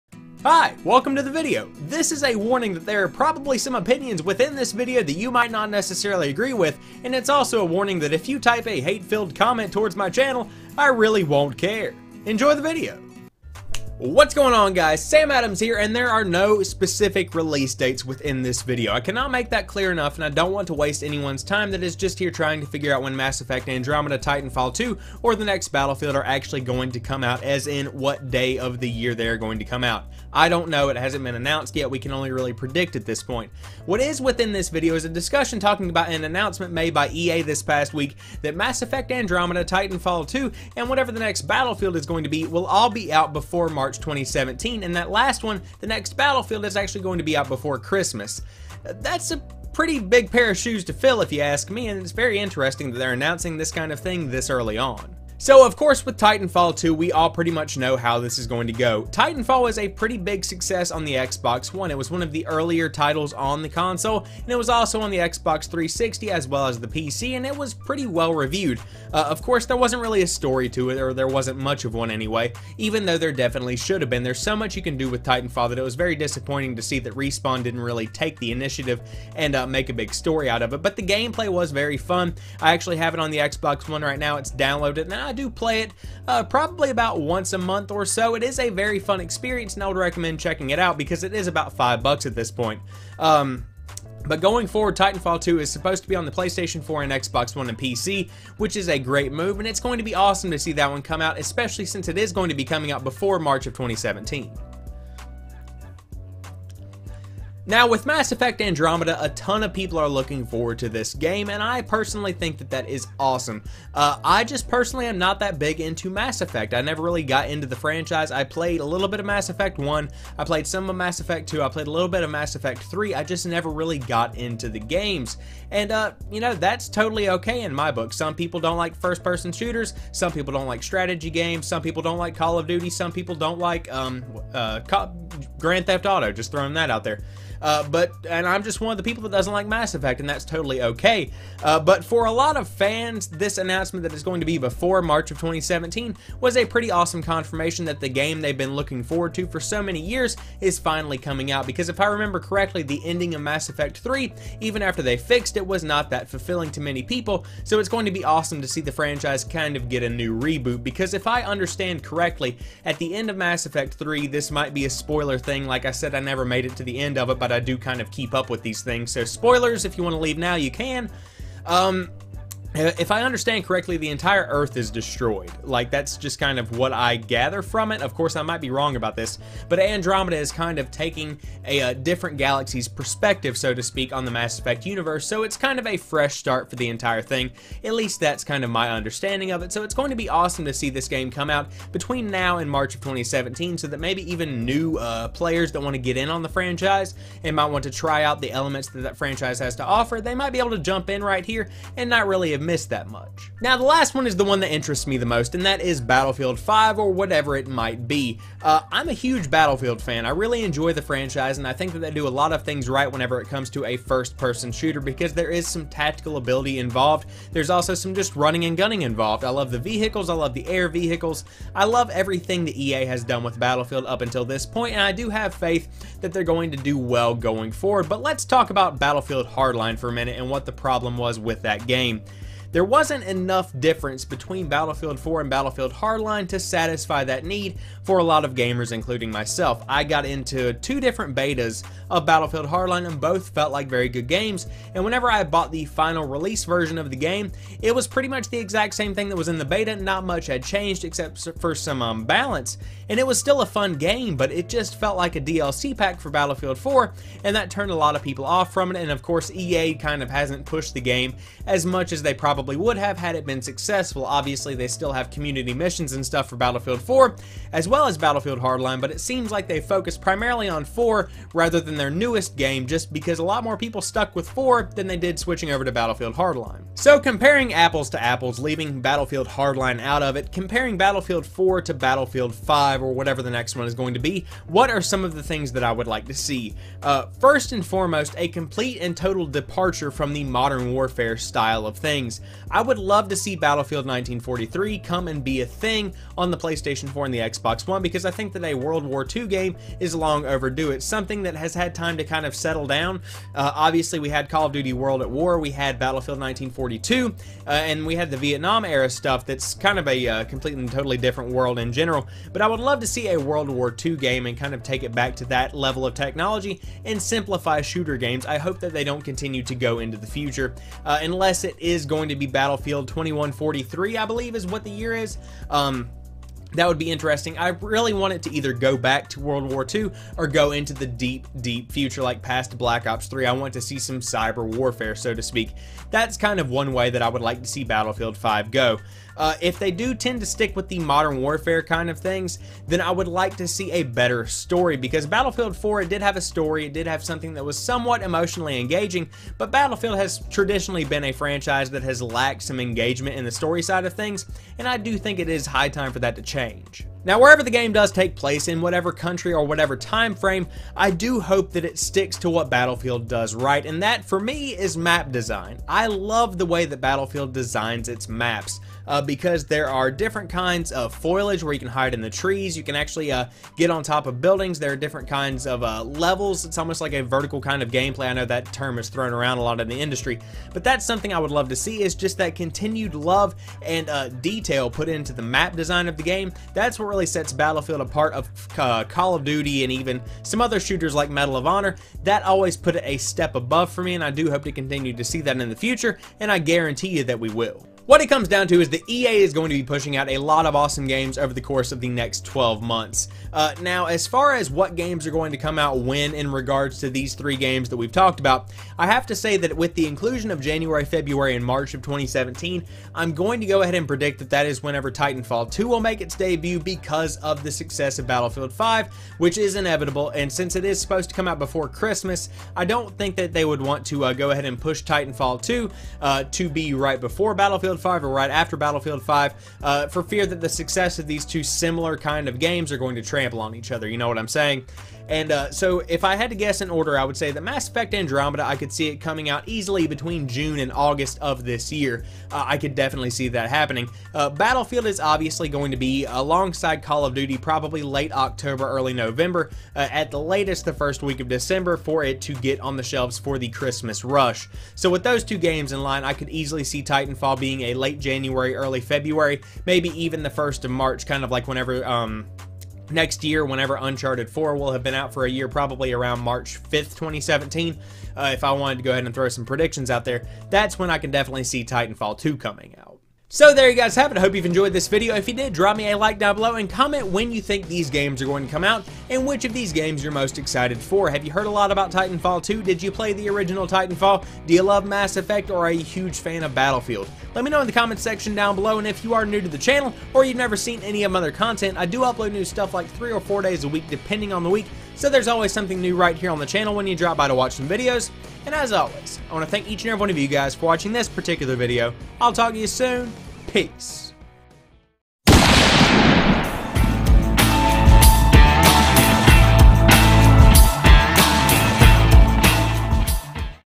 New Mass Effect, Titanfall, AND Battlefield by March 2017!
EA has been in the news a lot this week, but today we will be talking about their release dates! Mass Effect: Andromeda, Titanfall 2, and the next Battlefield are all slated to be out by March 2017! That is a big statement, so let's talk about the games and the announcement!

Want a new PC game? Use my link on GMG! Outro Music by Chuki Music Twitch